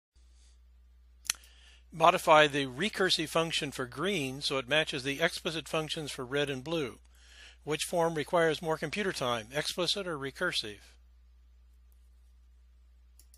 Math Machines Challenge 5
Modify the recursive function for green so it matches the explicit functions for red and blue. Which form requires more computer time, explicit or recursive?